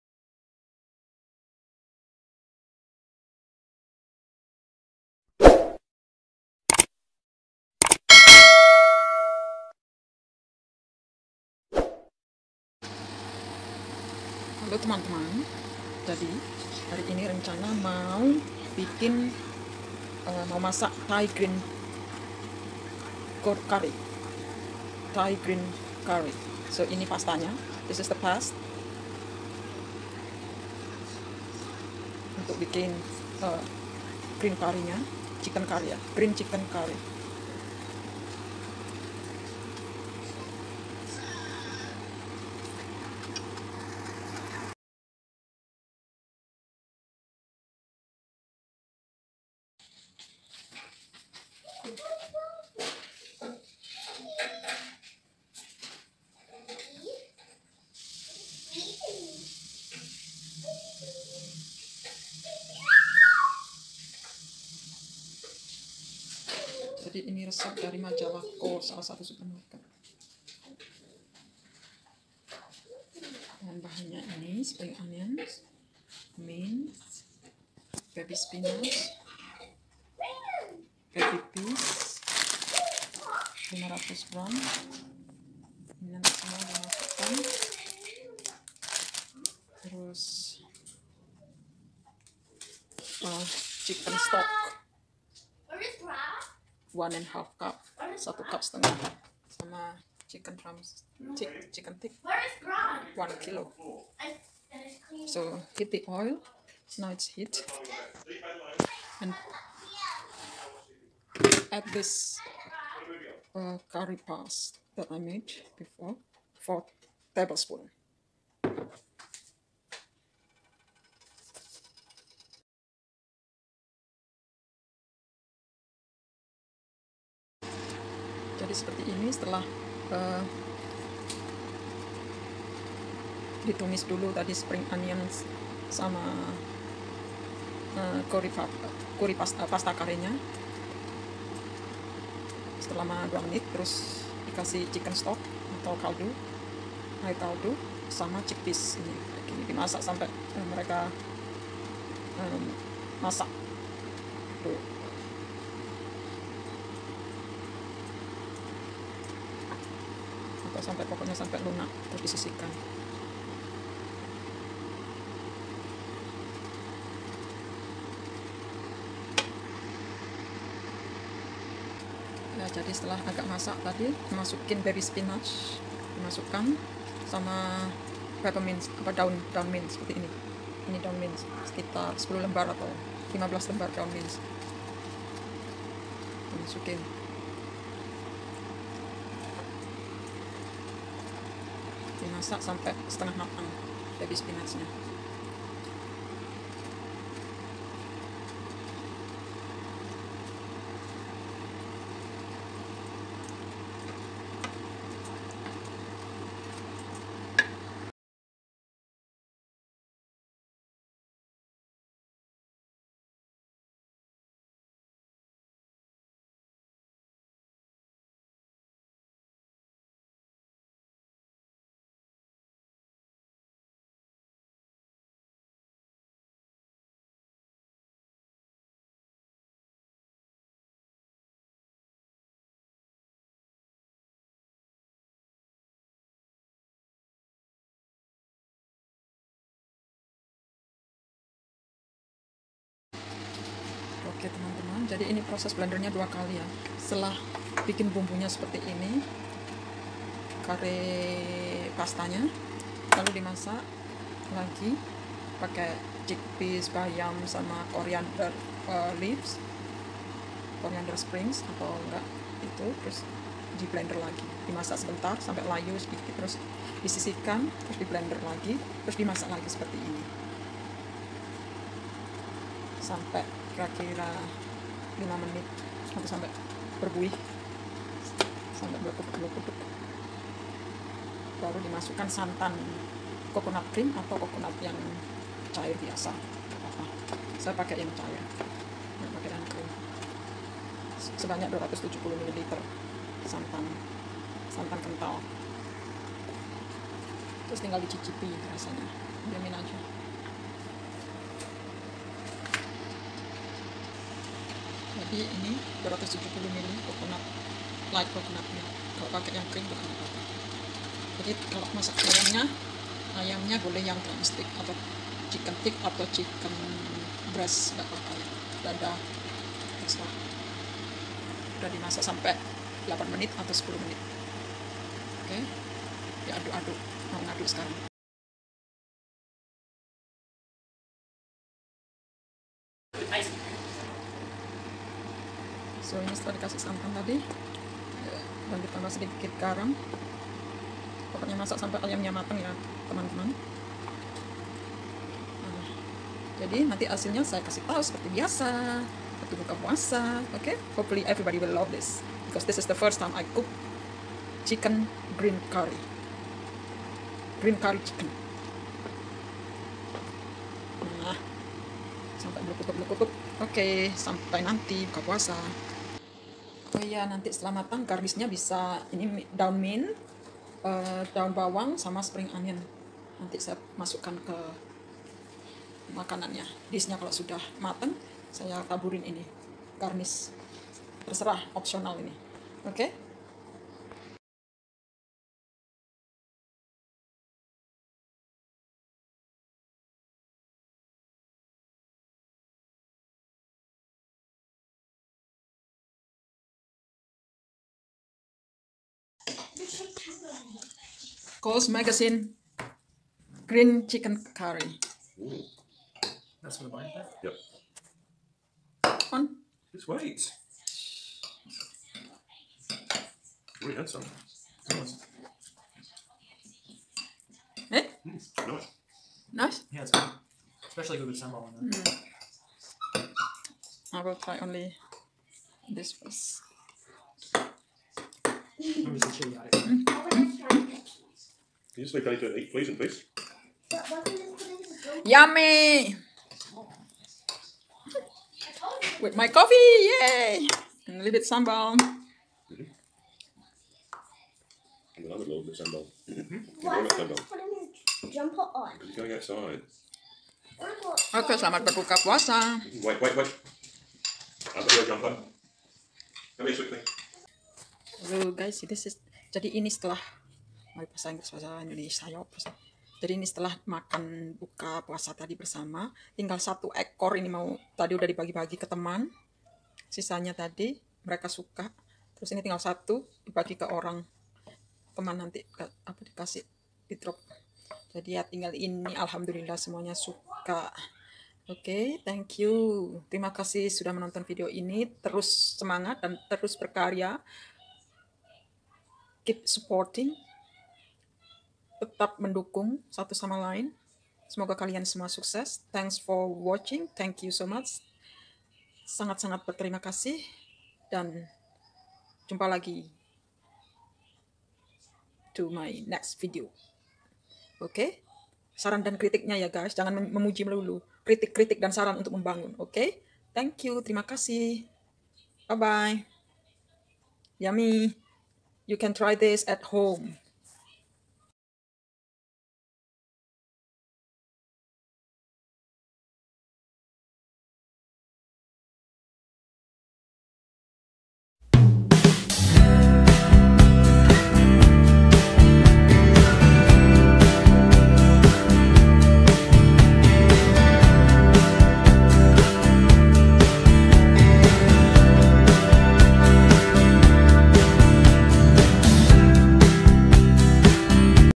Green Pea Chicken Curry - Resep yang sangat aneh!
Resep yang aneh menurut saya! Karena menuruti permintaan saya mencontoh dari majalah salah satu supermarket di Australia namanya Coles, edisi majalah May 2020!
Green pea chicken curry adalah makanan Thailand. Pasta curry (bumbu kare pastanya) bisa bikin sendiri atau boleh juga beli instant di supermarket, sesuai selera teman - teman.

- Jika mau membikin pasta geen curry sendiri resepnya klik disini (if you want to make your own green curry paste, click here):